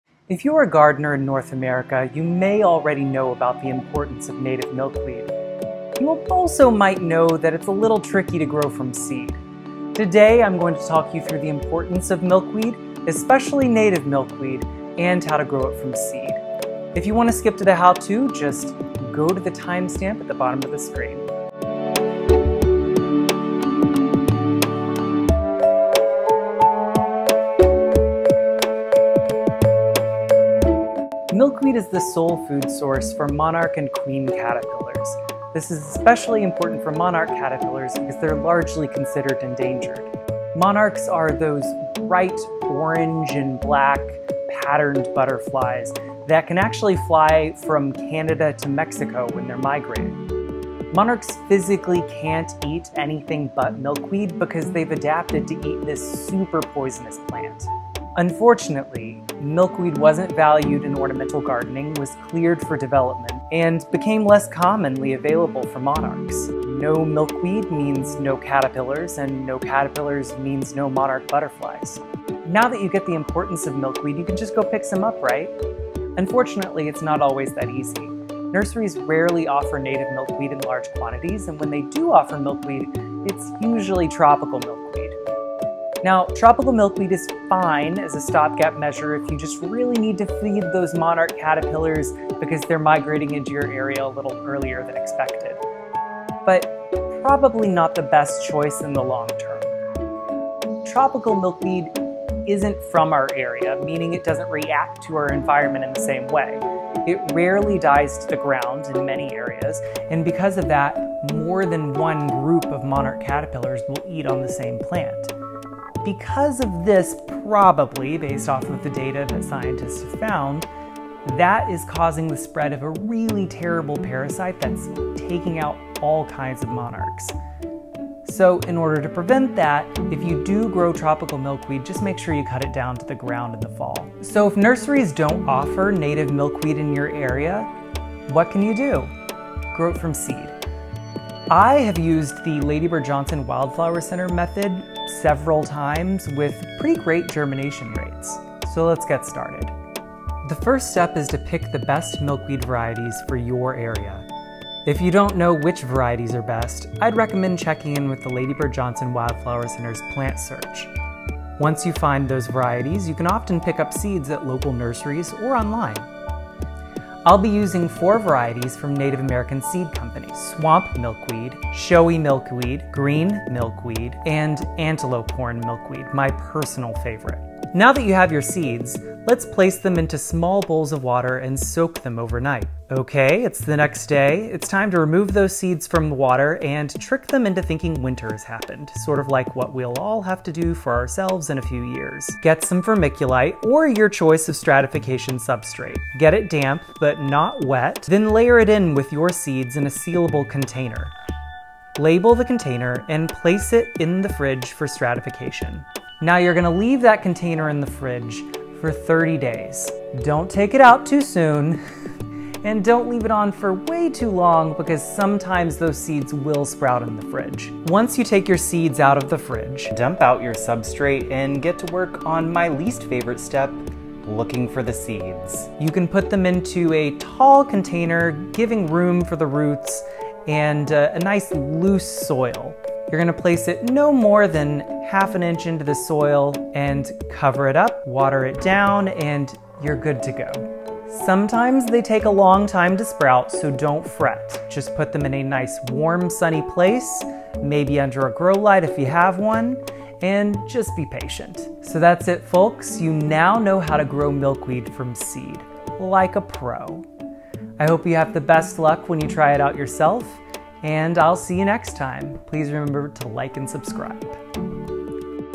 Growing Native Milkweed From Seed!
2:30 - Skip to How-To

Growing native milkweed is crucial to saving Monarch butterflies, but where the heck do you get it? Growing it from seed is easier than it sounds! We’re talking the importance of milkweed for Monarchs and how you can grow it from seed like a pro!